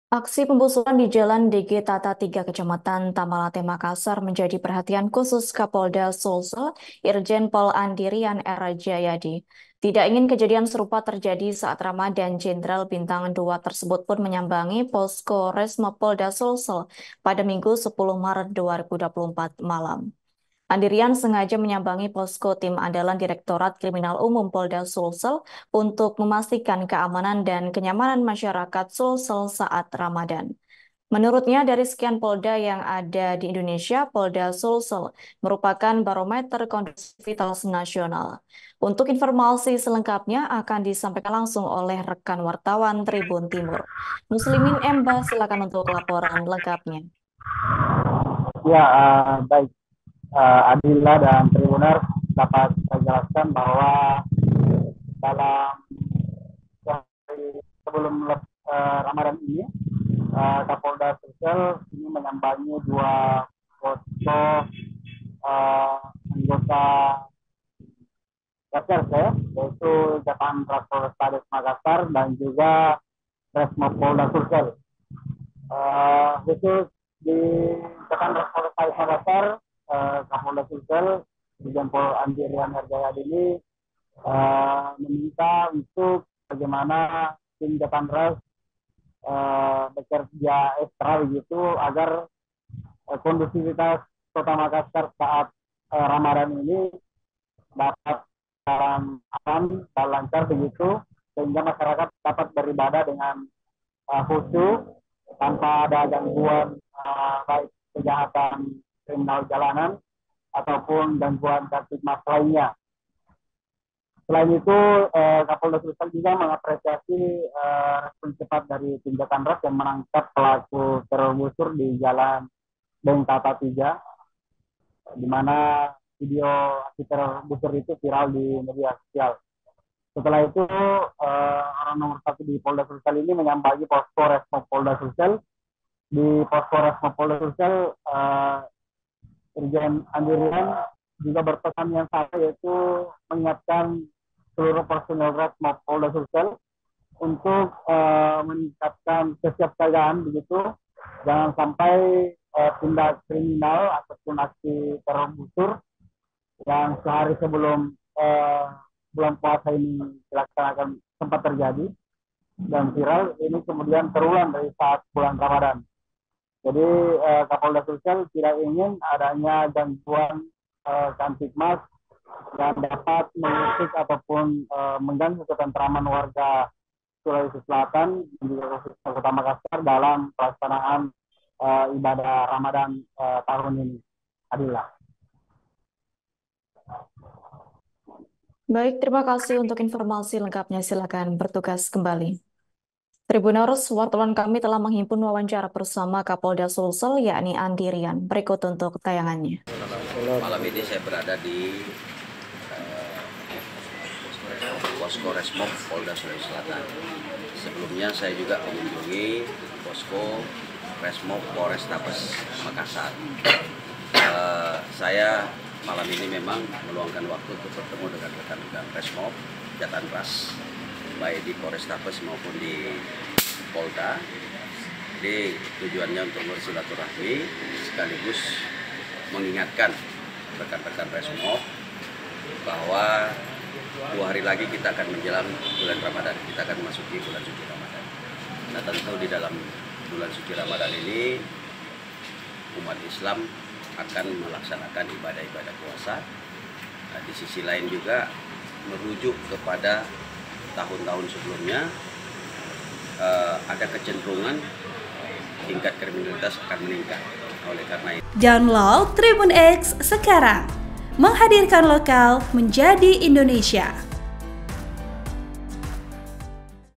Kapolda Sulsel ke Resmob untuk Pastikan Keamanan & Kenyamanan Warga Selama Bulan Ramadhan
Laporan wartawan Tribun Timur - Muslimin Emba
TRIBUN-VIDEO.COM- Aksi pembusuran di Jl Dg Tata 3, Kecamatan Tamalate, Makassar, menjadi perhatian khusus Kapolda Sulsel Irjen Pol Andi Rian R Djajadi.

Tidak ingin kejadian serupa terjadi saat Ramadan, jenderal bintang dua ini pun menyambangi Posko Resmob Polda Sulsel, Minggu (10/3/2023) malam.

Andi Rian sengaja menyambangi Posko tim andalan Direktorat Kriminal Umum (Ditkrimum) Polda Sulsel untuk memastikan keamanan dan kenyamanan masyarakat Sulsel saat Ramadan.

Menurutnya, dari sekian Polda yang ada di Indonesia, Polda Sulsel merupakan barometer kondusifitas nasional. (*)